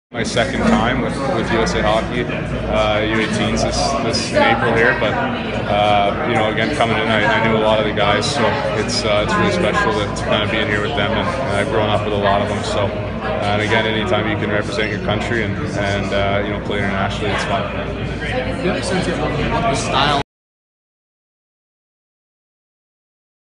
Logan Brown at USA Hockey summer camp
Although he originally sought to play for Canada, Windsor Spitfires forward Logan Brown says it feels right playing for USA Hockey.